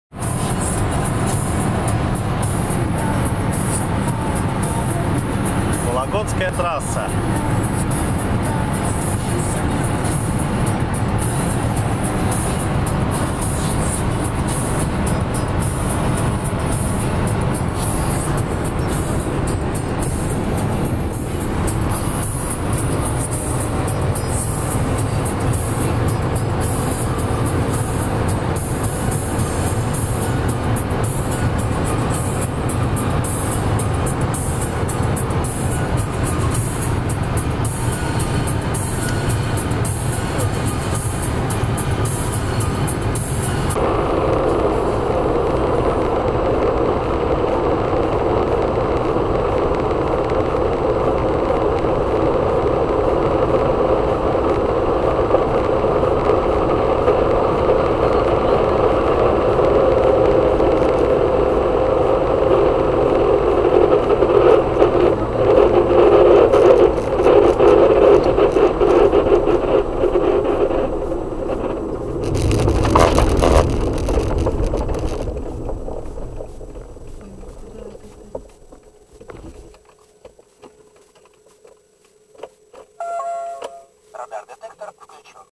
Legnum. 6a13 engine failed. Ушатал свой V6 двигатель
Двигатель застучал незаметно. Я прозевал этот момент. Когда разогнал машину на 180 км/ч - заметил, что двигатель вместо 4-й передачи и 5 тыс.оборотов, идёт на 6,2 тыс.об, на 3-й передаче. Потом увидел стрелку температуры выше нормы. Сбавил оборо и скорость, но было уже поздно - в движке пошли необратимые процессы по разрушению. Проехал ещё 30-40 км и двигатель заклинило. Этим моментом заканчивается видеоролик.